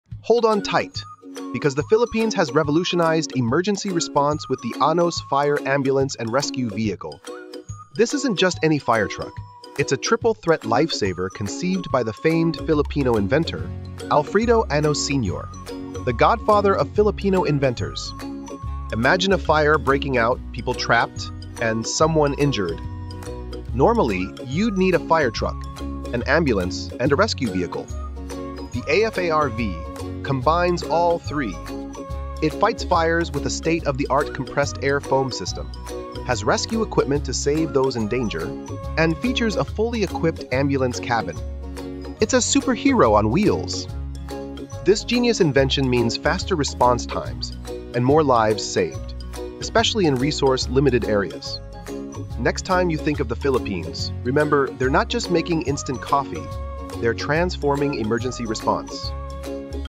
Discover the Filipino-Made Lifesaving Super Vehicle
This genius invention means faster response times and more lives saved, especially in resource-limited areas. Anos Sr.'s company, Anos Research Manufacturing, also produces innovative fire trucks like the "Penetrator" and the "Patriot." Next time you think of the Philippines, remember – they’re not just making instant coffee; they’re transforming emergency response!